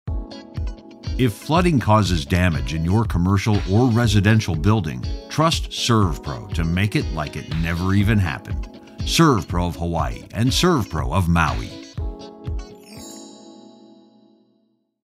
Hawaii│Maui Extracts Lake-Sized Amounts of Water, Restores Properties
If floods swamp your commercial or residential property, call our Hawaii│Maui team ASAP for high-quality restoration services!